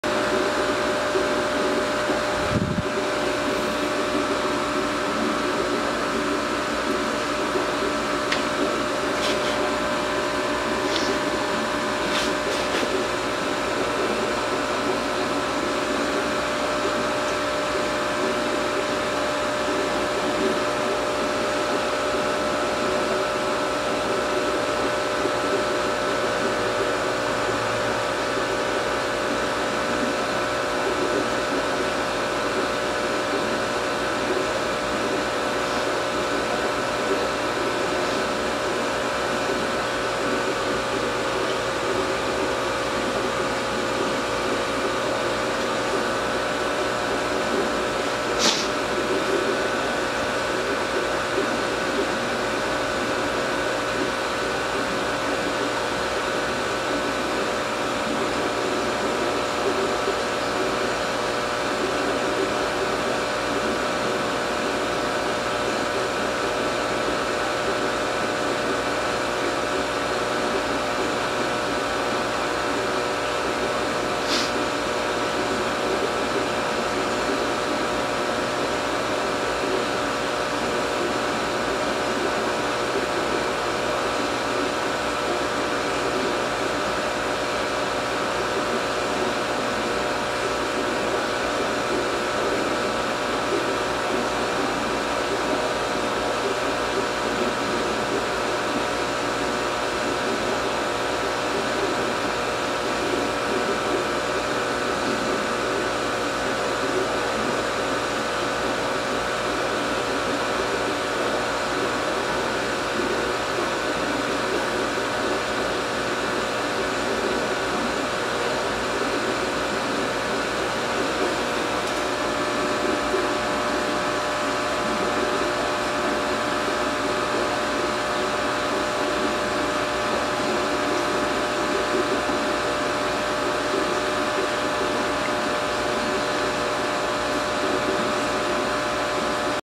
Longnose gar(Lepiosteus osseus) Hikari arowana feeding
my Longnose garfish(Lepiosteus osseus) Hikari arowana feeding